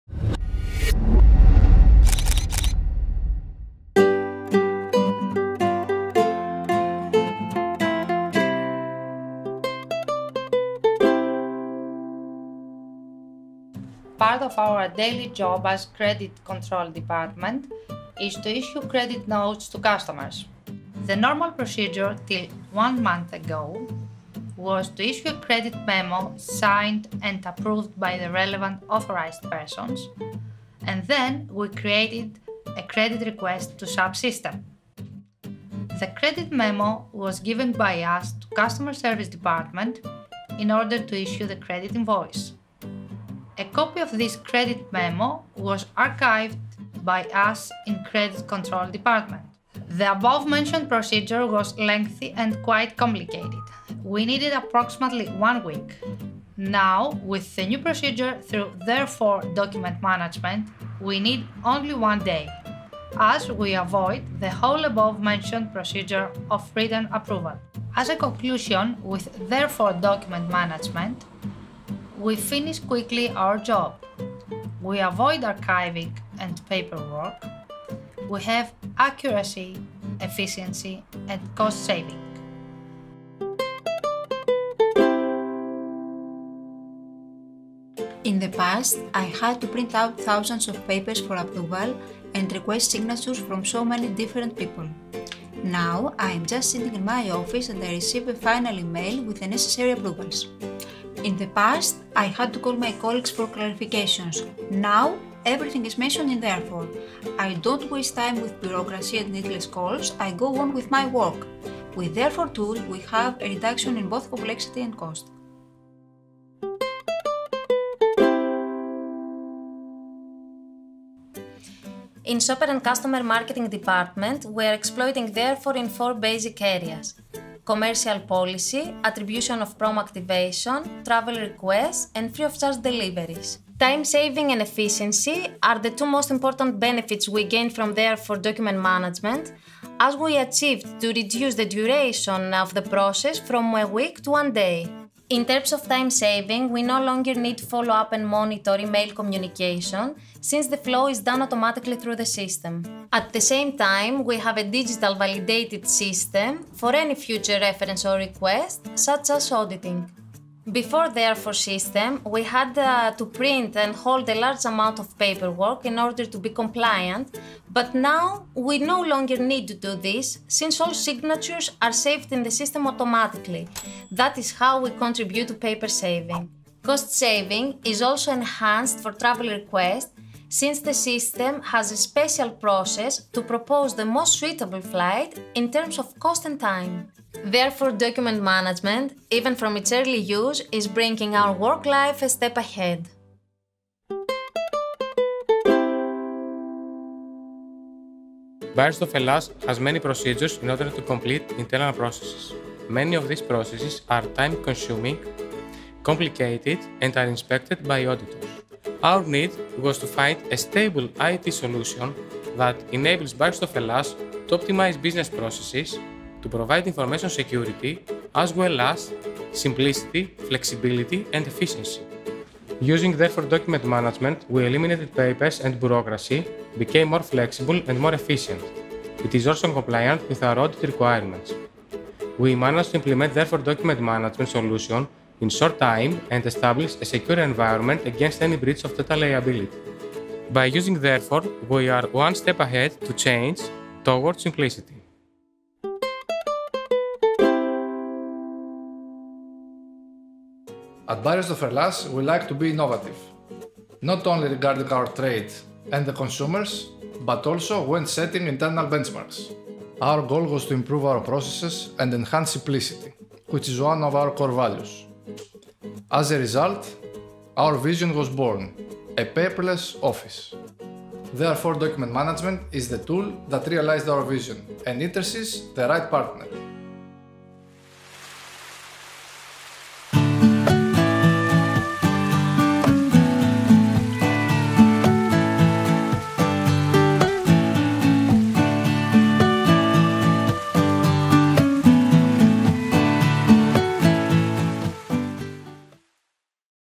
Case Study: Beiersdorf Hellas uses Therefore™
Beiersdorf is an international skin care company with globally leading brands such as NIVEA, Eucerin, La Prairie, Labello and Hansaplast. The company’s branch in Greece uses Therefore™ to issue credit notes to customers. This saves time and costs, increases the credit control department’s accuracy and efficiency, and reduces the amount of paper work. The shopper and customer marketing department has profited, as well. By installing Therefore™, they have improved several key work processes.